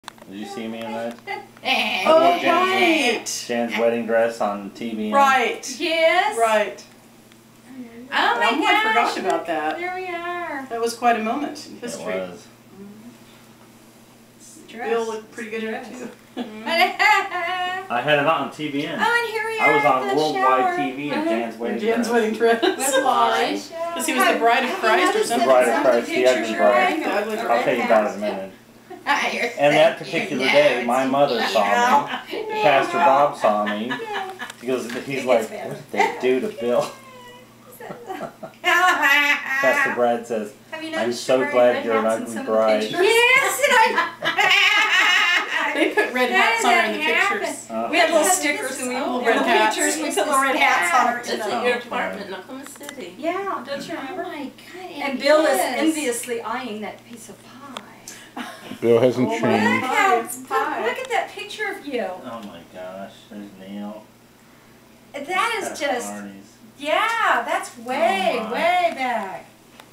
Jan Miller Oregon coast July 2008
Jan Miller at the Oregon coast in July 2008, enjoying her birthday gift of a photo album of old school photos after being "red hatted" for her 50th birthday by her friends Gail, Cheryl & Suzanne.